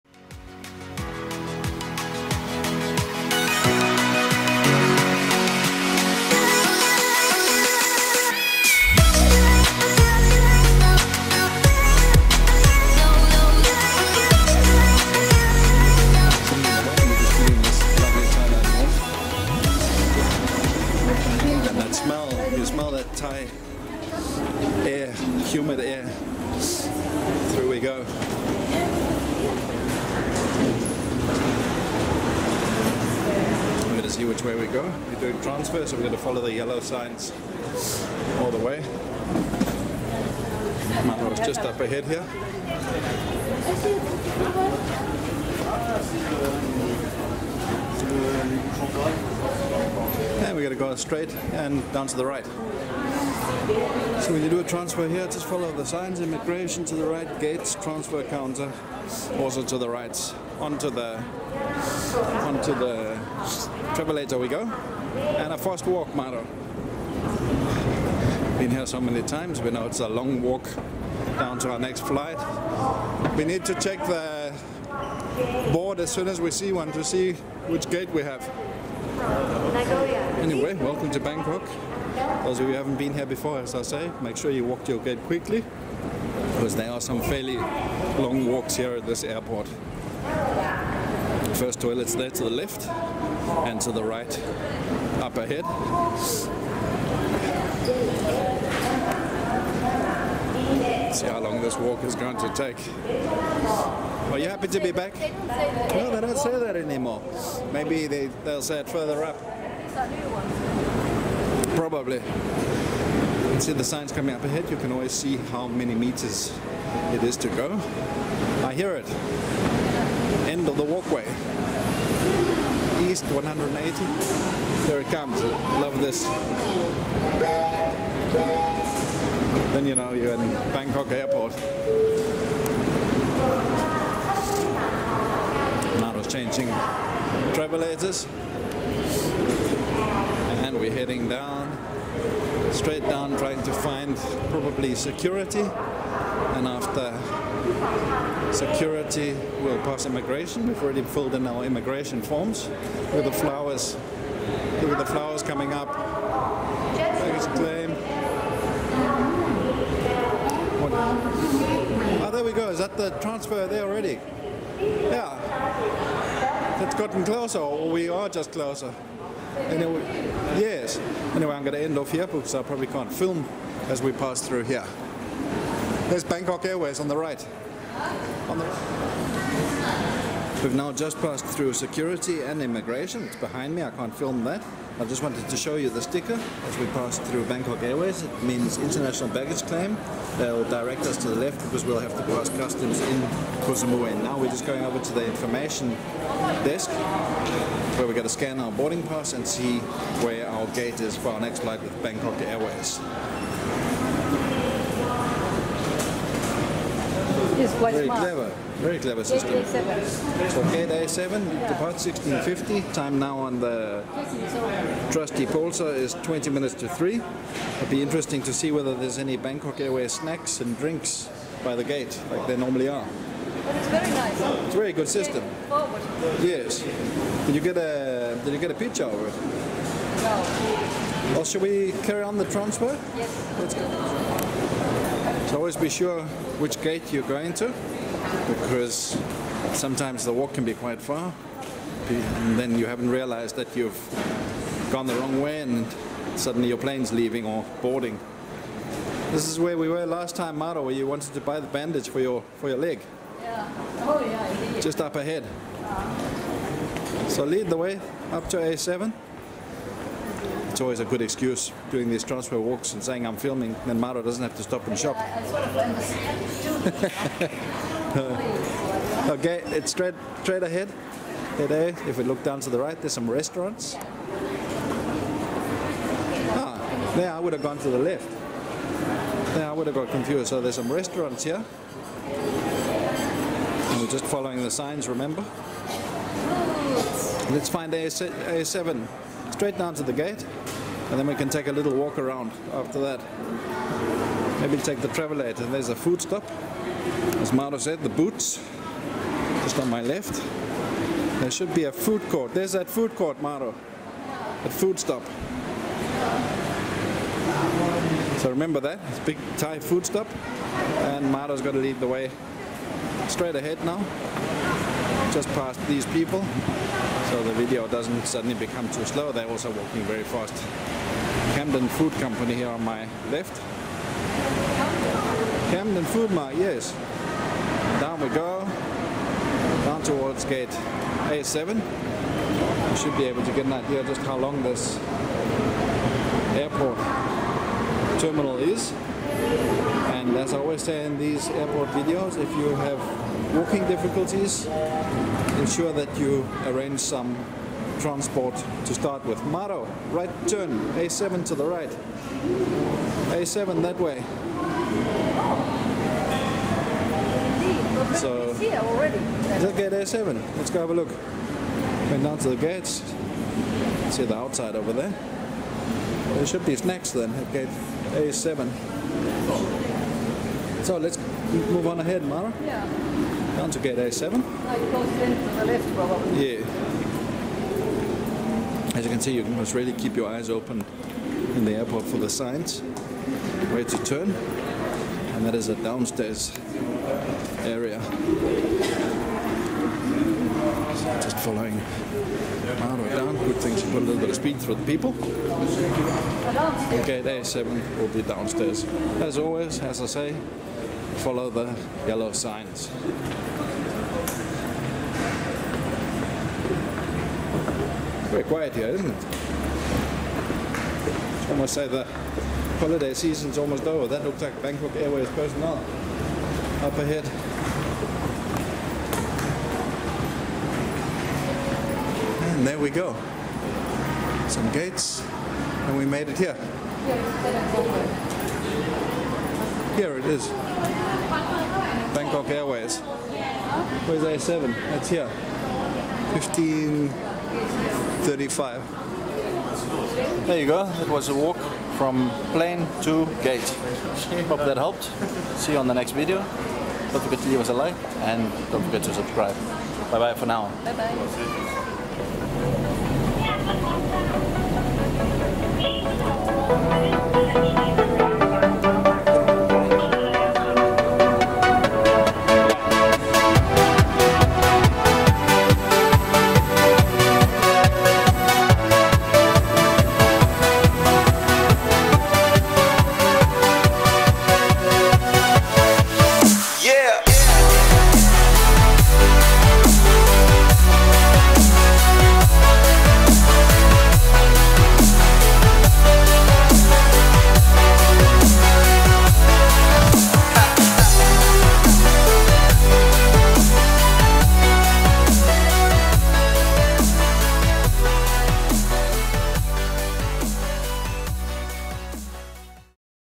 TRANSFER AT BANGKOK Suvarnabhumi Airport - Connection flight at Bangkok Airport
We have just arrived at Bangkok Airport officially called Suvarnabhumi Airport on a Lufthansa flight from Frankfurt. We now have to make our way through Bangkok Airport to find the Gate for our next flight with Bangkok Airways to Koh Samui. This flight Transfer Video shows passengers what it is like to have a Flight Transfer at Bangkok Suvarnabhumi Airport and what they can expect. Transfer walks can be very long at Suvarnabhumi Airport or in this case the Transfer walk was rather short.

Our Flight Connection videos are filmed in (almost) real time so that you get a good idea of how long the walk can take. Transfer walk times can differ from time to time as it depends on how many passengers are walking in front of you, passing through Airport Security and passing through Immigration / Passport Control.

Wiki writes about Suvarnabhumi Airport, "Suvarnabhumi Airport (IATA: BKK, ICAO: VTBS), also known as (New) Bangkok International Airport, is one of two international airports serving Bangkok, Thailand. The other one is Don Mueang International Airport. Suvarnabhumi covers an area of 3,240 hectares (8,000 acres), making it one of the biggest international airports in Southeast Asia and a regional hub for aviation.
Suvarnabhumi was officially opened for limited domestic flight service on 15 September 2006, and opened for most domestic and all international commercial flights on 28 September 2006..."

This Suvarnabhumi Airport Transit Walk to a Connection Flight video is filmed at Suvarnabhumi Airport, also called Bangkok International Airport.

CopenhagenInFocus (Our at home Profile)

Your support helps us bring you more videos like this.